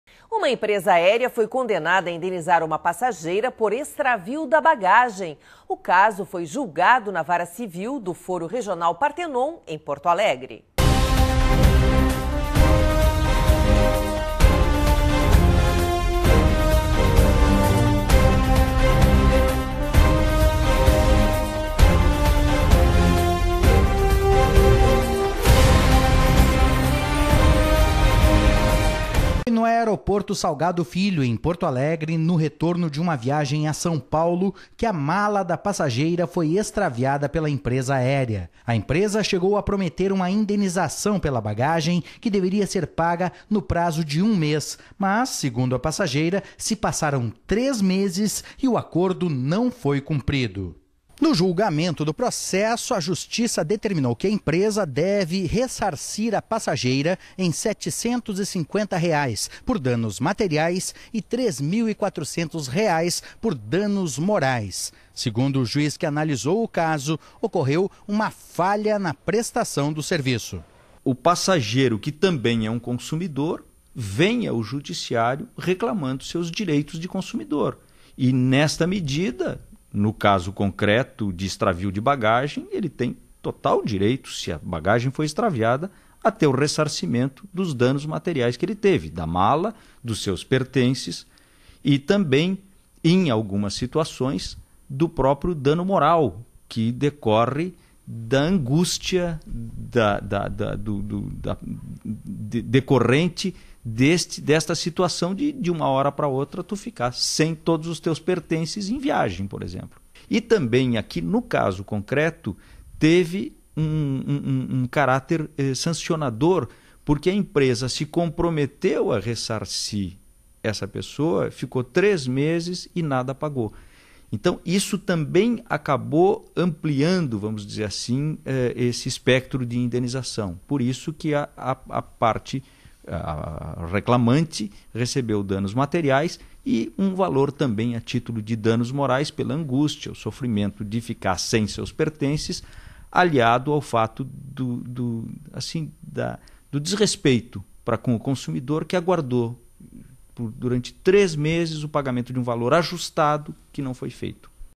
Empresa aérea é condenada a indenizar passageira por extravio de bagagem
Justiça Gaúcha nº 196: Empresa aérea é condenada a indenizar passageira por extravio de bagagem